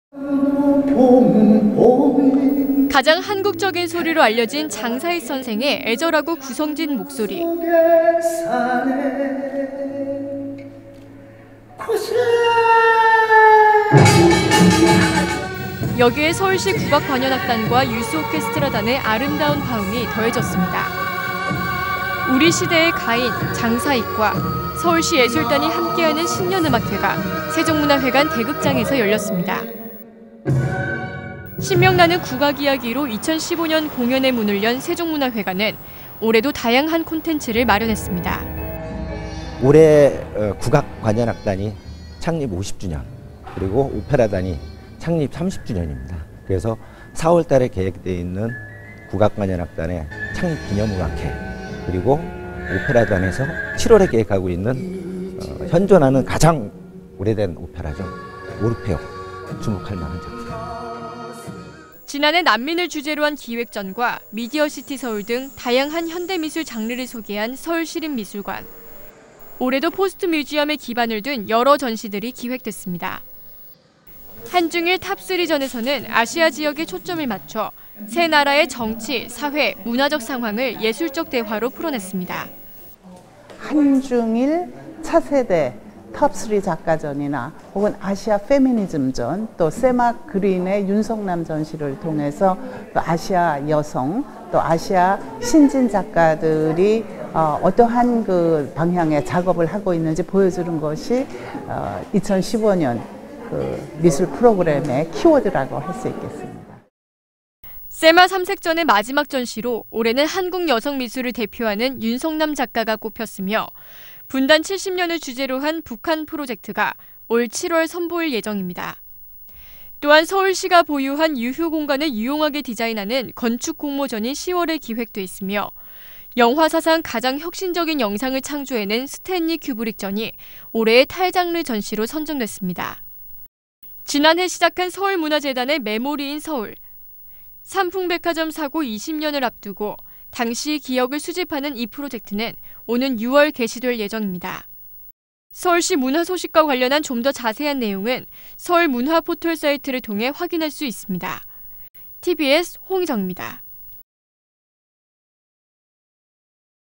새해 주목해 볼 공연·전시
새해 주목해 볼 공연 전시는 어떤 것이 있을까요? 서울시 산하 여러 문화 기관들이 올 한 해도 시민들이 즐길 수 있는 다양한 작품 공연들을 야심차게 준비했습니다. 올해는 어떤 작품들을 만나볼 수 있는지 정리해봤습니다.